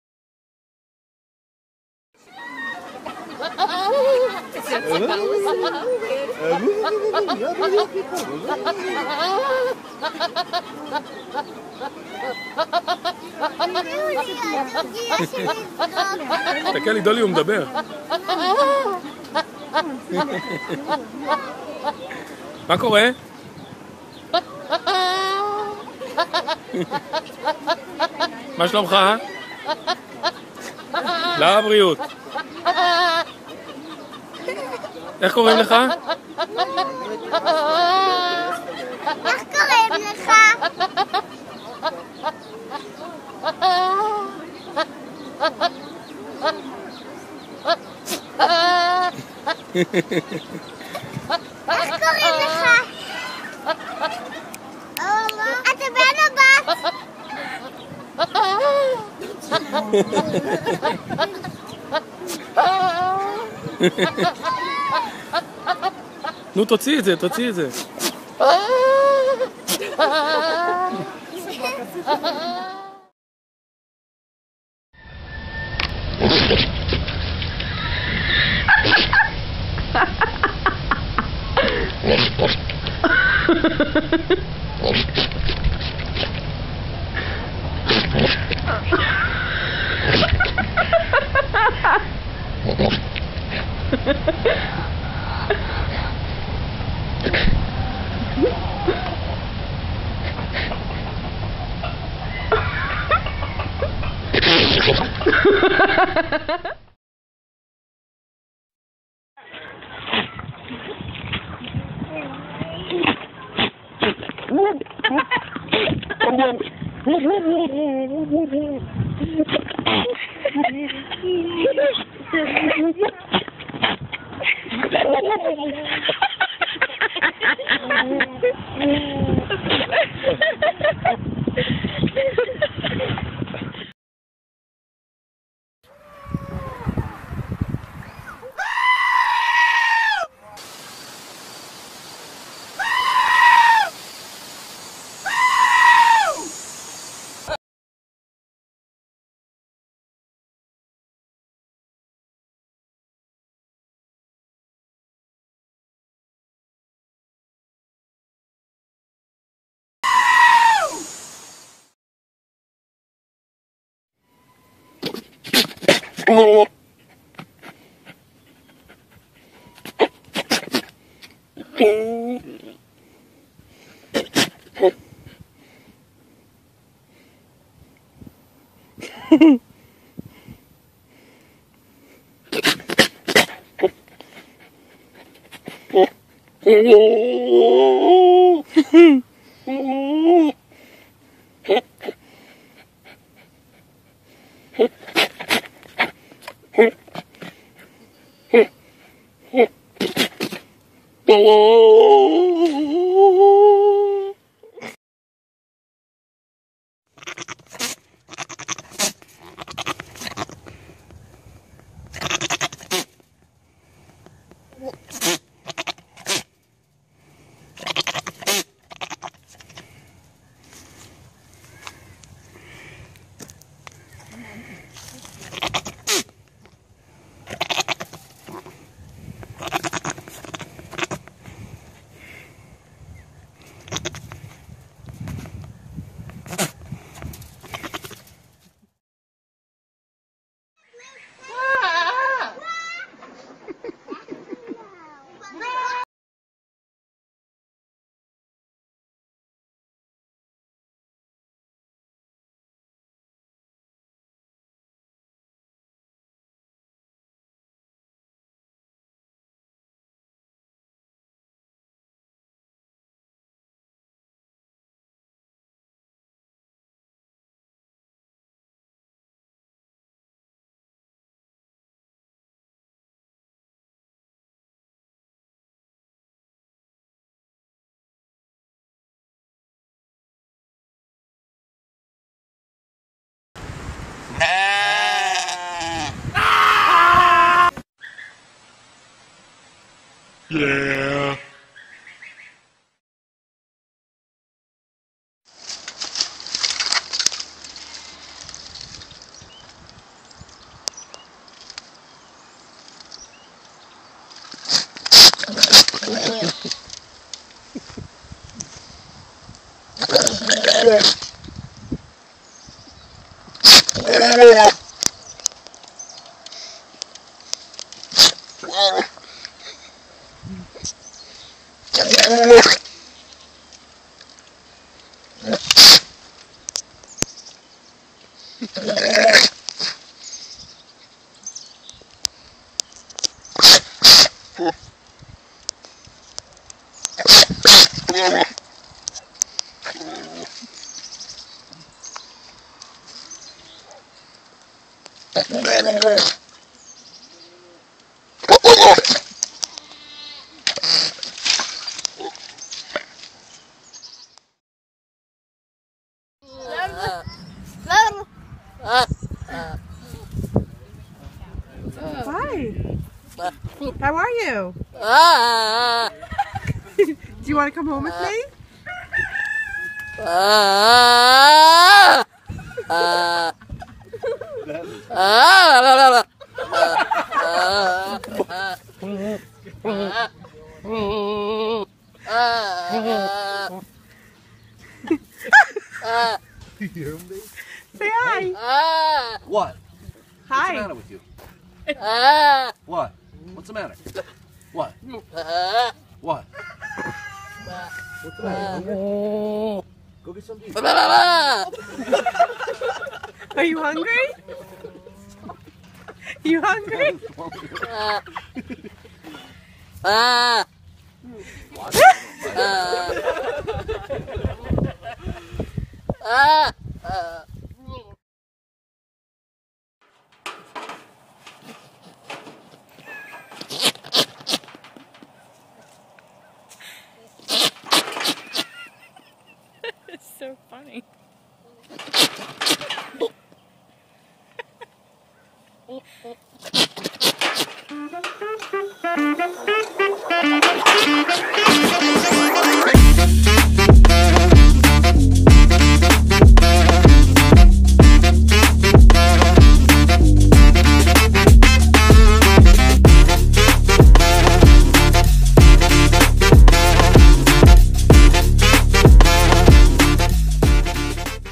Top 10 Funny And Wierd GOATS Noise
The funniest and funniest scenes I saw of goats and I couldn't stop. I have prepared the top 10 funniest goat sounds for you and I hope you enjoy the whole video. Thank you for your support, we will raise the channel and I will upload more great videos.